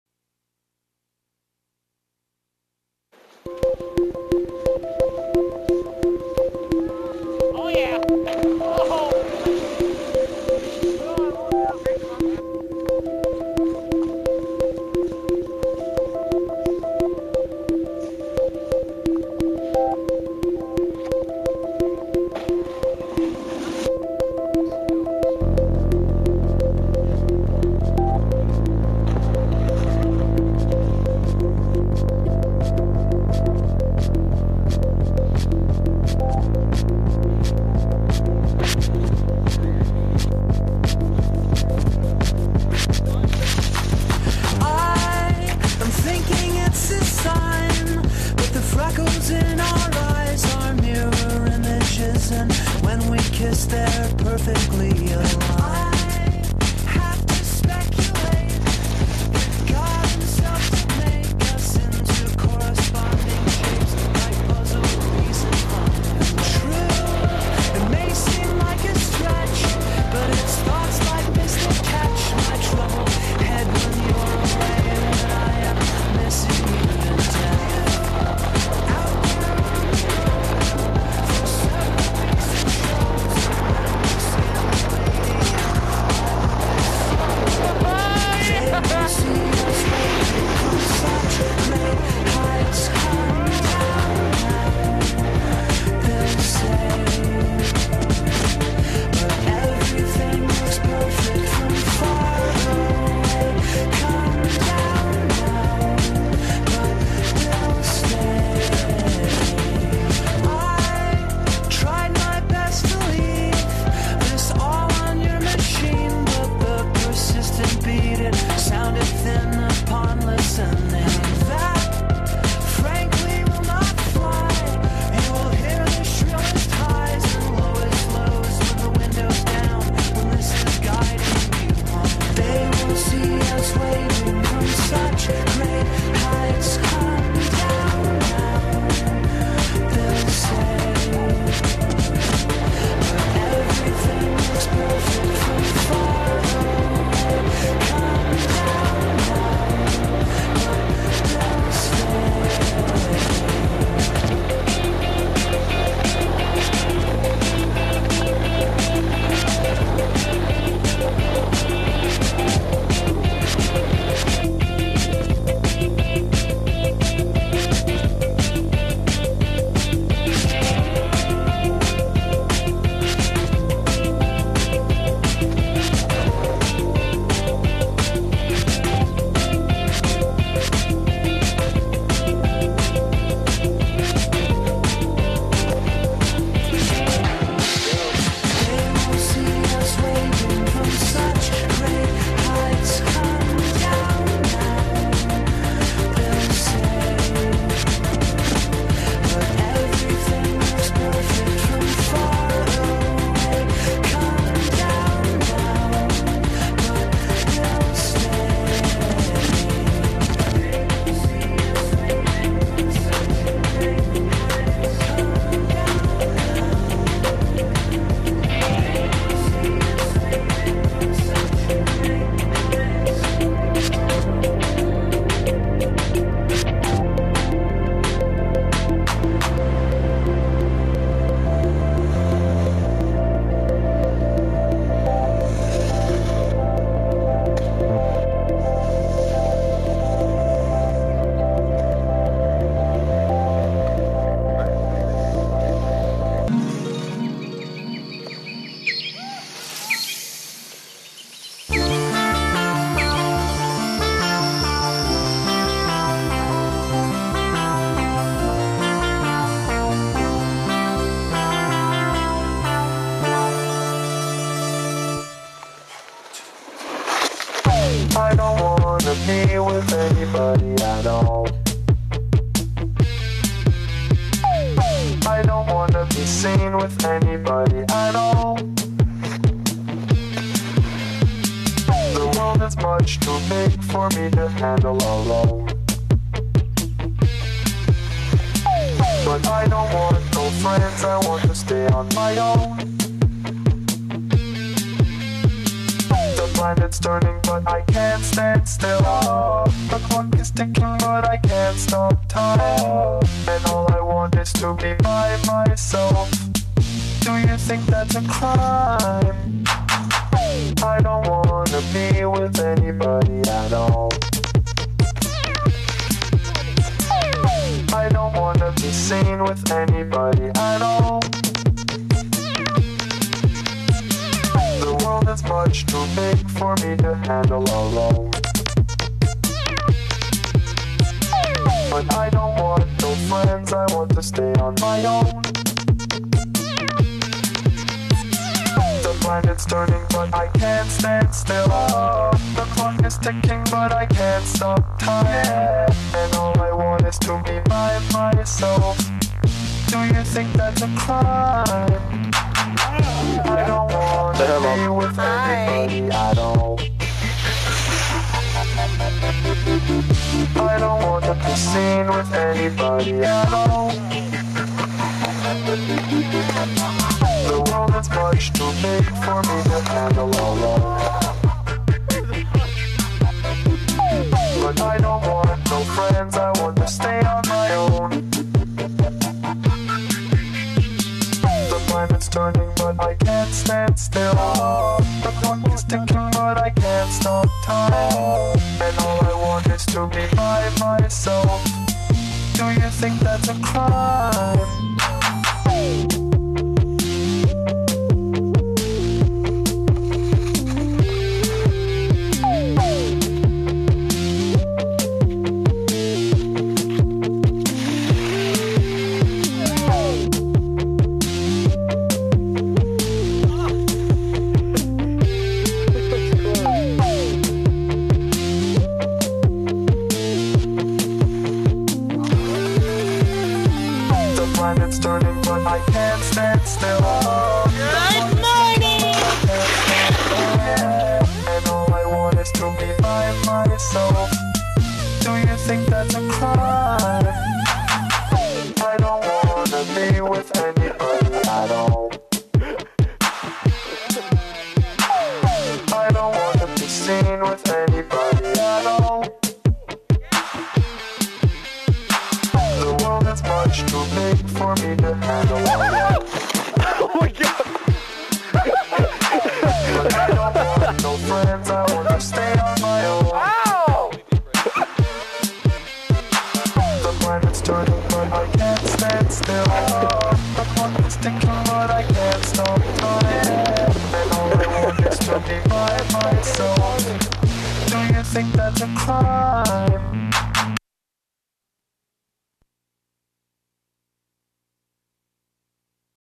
Soggy Waffles - Film Fest
Mike Mankosa won the 3rd Prize $50 gift card. His film showcased some awesome action at the Peak.